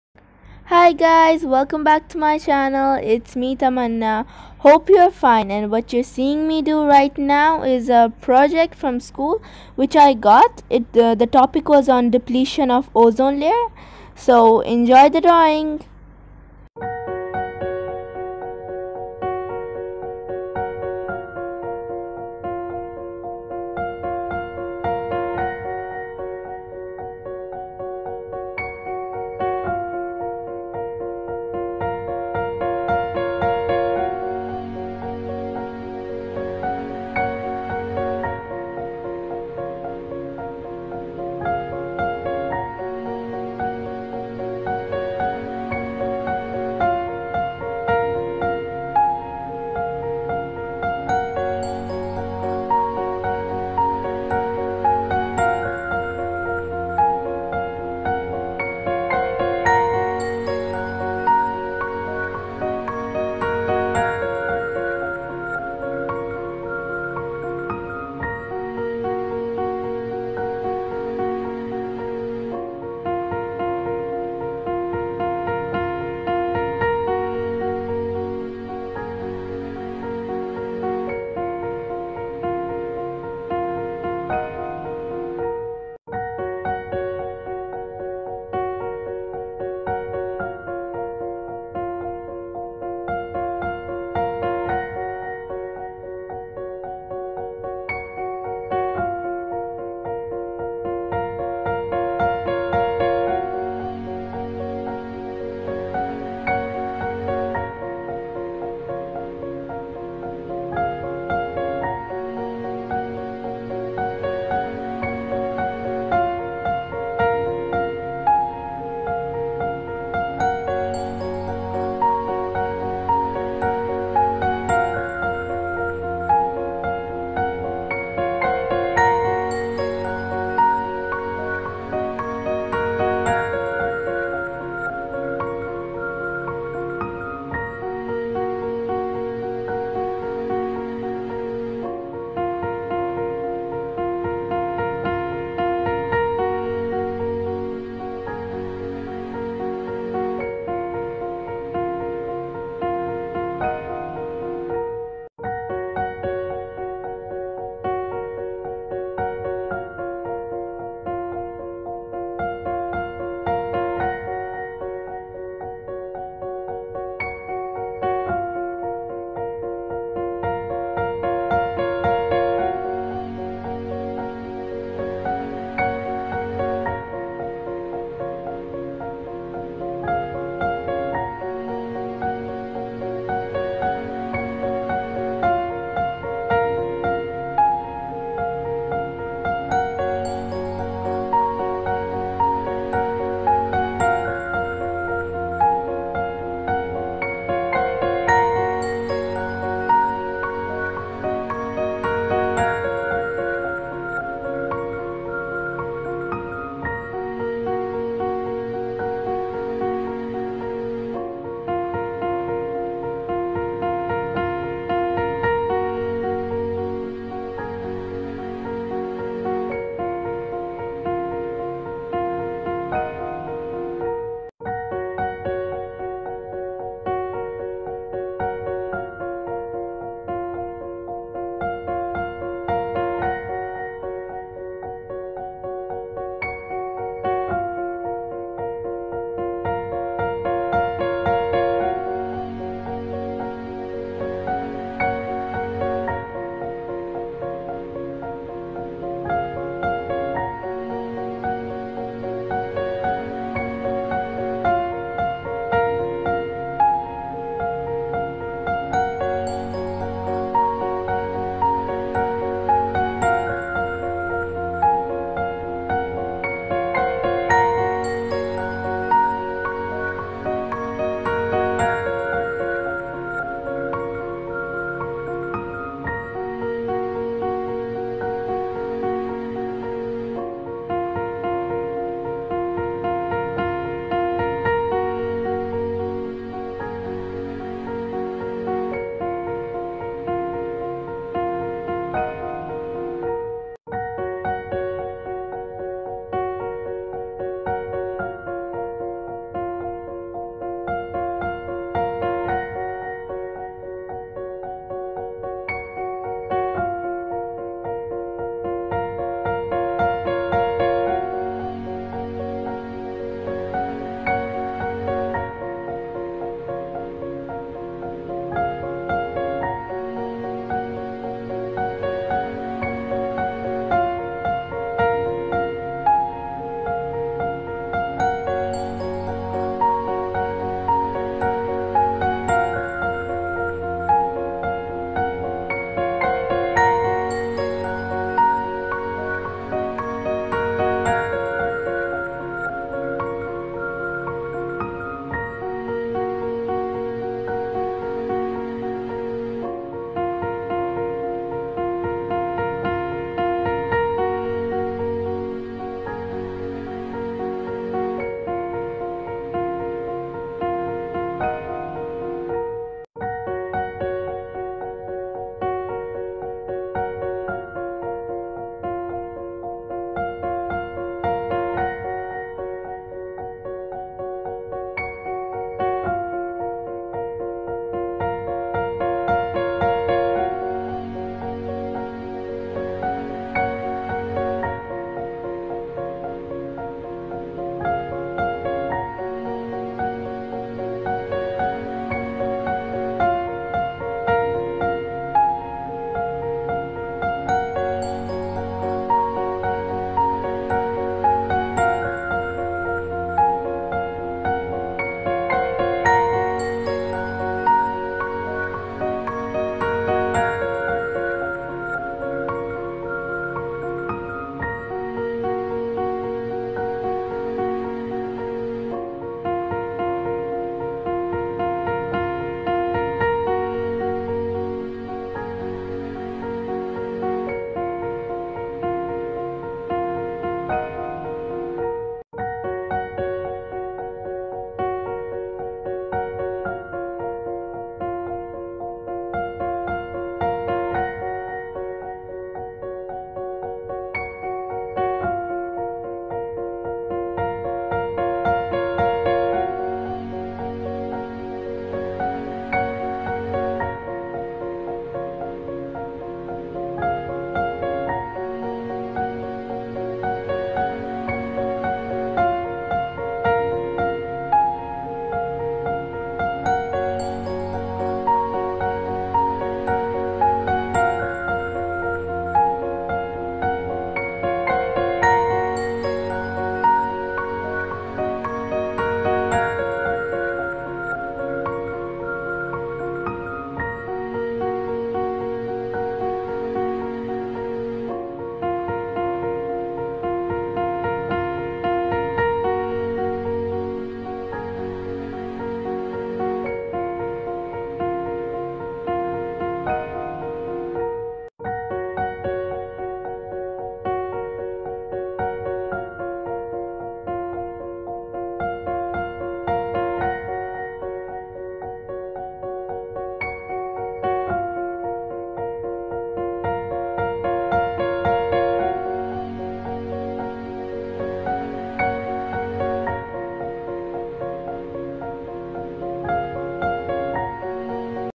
Drawing Of Dipletion Of Ozone Layer!!!
In this video I draw Dipletion Of Ozone Layer which is a project from my school.